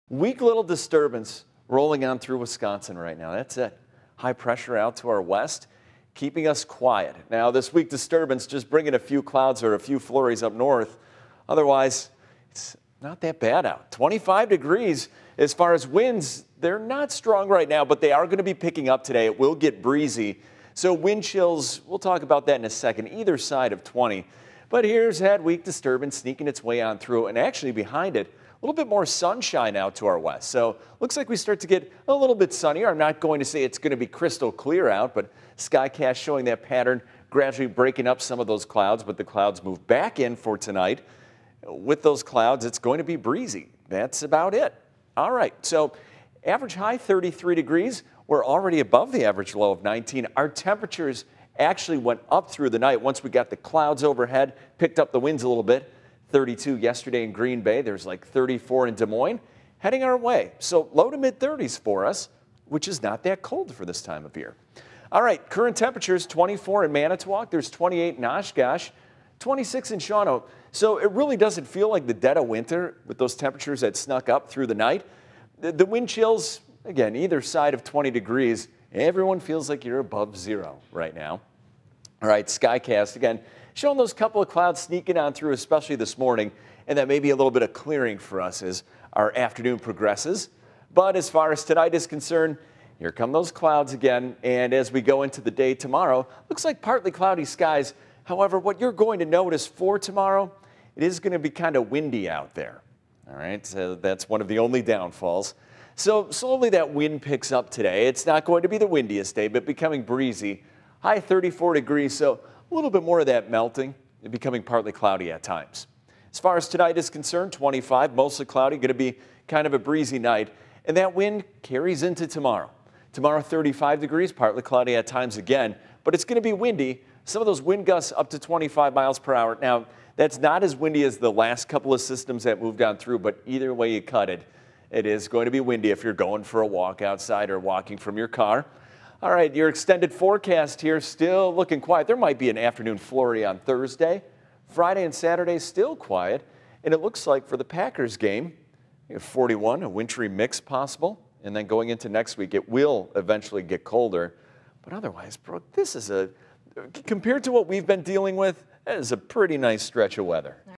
Michael Fish's NBC26 weather forecast
We're going to have another quiet day, though it will be kind of breezy. Highs will top out in the mid-30s.  Tonight will also be breezy and quiet with mild lows in the mid-20s.  Wednesday it will be kind of windy, but still dry. Highs will once again be in the mid-30s.  There's a chance of a few afternoon flurries on Thursday and Friday will be colder.  A dry stretch of weather carries over into the first half of the weekend with no significant snow or rain currently anticipated. Sunday, there is a chance of a light wintry mix/rain with highs around 40.